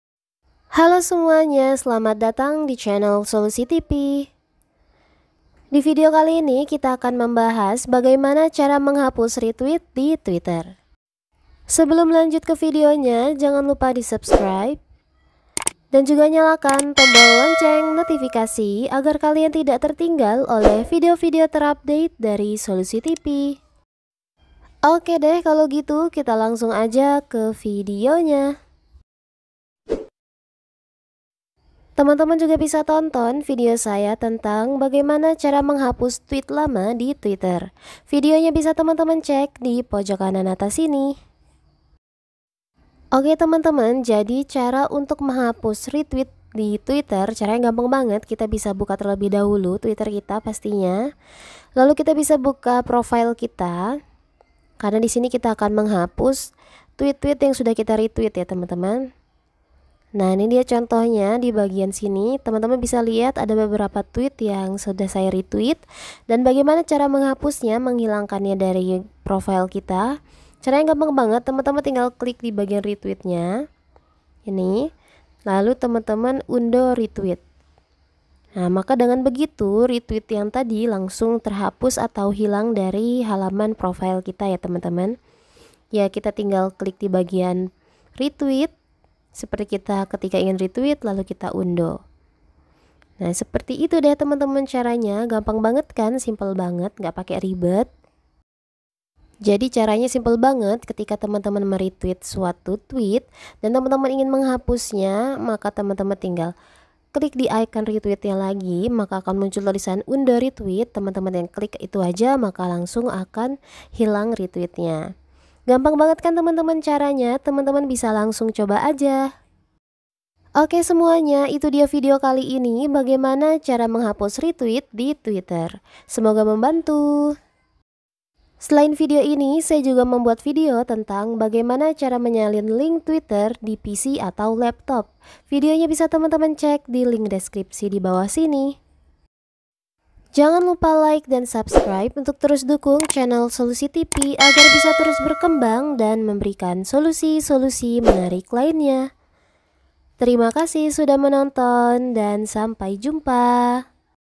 Cara Hapus Retweet di Twitter || Retweets Twitter
Bagaimana cara membatalkan atau menghapus Retweet saya?
Kalian dapat membatalkan Retweet yang telah dibuat dengan mengeklik ikon Retweet yang disorot di Tweet. Dengan cara ini, Retweet pasti terhapus dari timeline kalian, tetapi Tweet asli tidak akan terhapus.